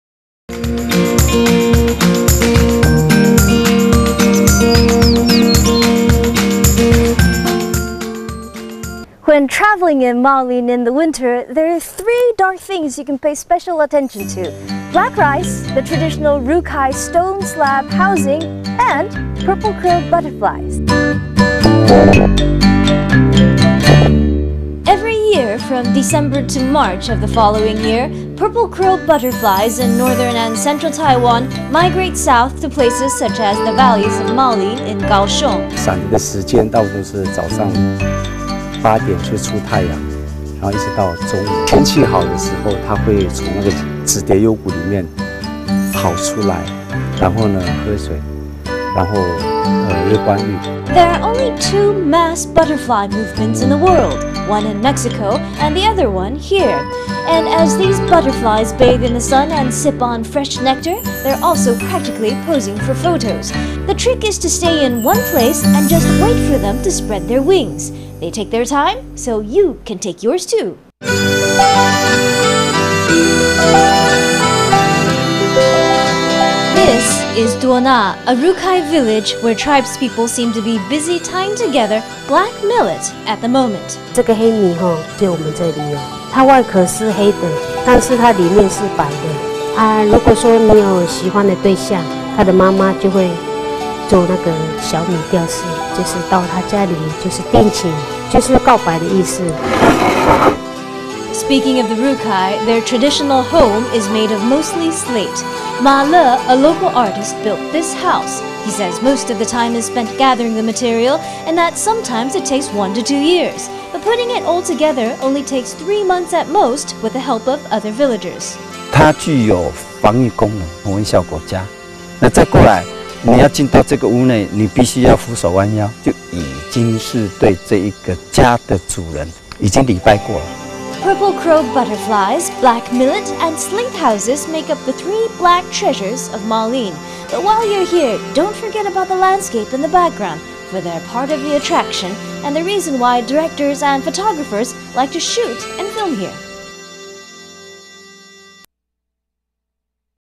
Maolin's Three Black Treasures.mpg
When traveling in Maolin in the winter, there are three dark things you can pay special attention to: black rice, the traditional Rukai stone-slab housing, and purple crow butterflies.

Every year from December to March of the following year, purple crow butterflies in northern and central Taiwan migrate south to places such as the valleys of Maolin in Kaohsiung.

Chen Cheng, Butterfly Expert:
The best time to watch butterflies is from 8am when the sun is up until noon. When the weather is nice, they'll come out of the valleys to drink water and get some sun.

There are only two mass butterfly movements in the world - one in Mexico and the other here. And as these butterflies bathe in the sun and sip on fresh nectar, they're also practically posing for photos. The trick is to stay in one place and just wait for them to spread their wings. They take their time, so you can take yours too.

This is Duona, a Rukai village, where tribespeople seem to be busy tying together black millet at the moment.

Only we have black millet rice. They're black on the outside and white on the inside. When a man falls in love with a woman, his mother will make these millet bouquets for him to bring to her house. It's a show of affection.

Speaking of the Rukai, their traditional home is made of mostly slate. Ma-le, a local artist, built this house. He says most of the time is spent gathering the material and that sometimes it takes one to two years, but putting it all together only takes three months at most with the help of other villagers.

Ma-le, Artist:
These are designed to keep invaders at bay, and to keep temperature levels from fluctuating. The low roof forces visitors to bow when entering the house, so as they're coming in, they're paying respect to the host at the same time.

Purple crow butterflies, black millet and slate houses make up the three "black treasures" of Maolin. But while you're here, don't forget about the landscape in the background, for they're part of the attraction, and the reason why directors and photographers like shoot and film here.